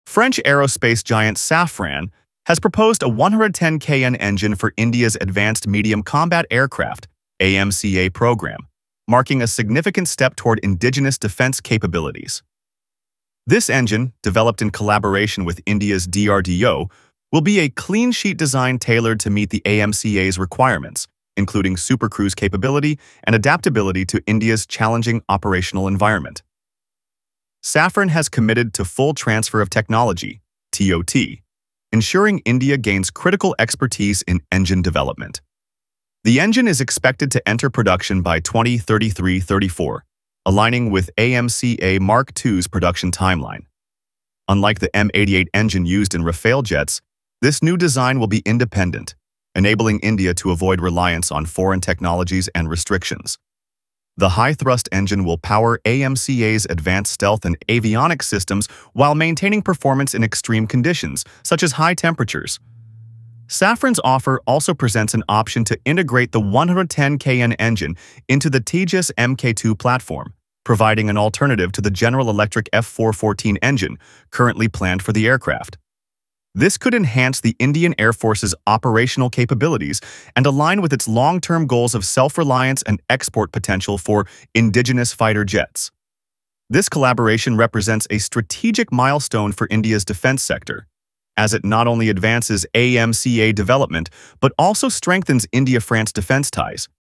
M88 Out? Safran Unveils Plans for Indigenous AMCA Engine..
Safran ने घोषणा की है कि उनका नया 110 kN इंजन पूरी तरह से भारत के AMCA (Advanced Medium Combat Aircraft) प्रोजेक्ट के लिए डिज़ाइन किया जाएगा, और यह फ्रेंच M88 तकनीक से पूरी तरह स्वतंत्र होगा। यह कदम भारत की रक्षा क्षेत्र में आत्मनिर्भरता की दिशा में एक बड़ा प्रयास है। नया इंजन अत्याधुनिक तकनीक और भारतीय लड़ाकू विमान की जरूरतों के अनुरूप होगा। Safran का यह विकास AMCA को एक नई उड़ान देने के साथ-साथ भारत के एयरोस्पेस सेक्टर में एक मील का पत्थर साबित हो सकता है।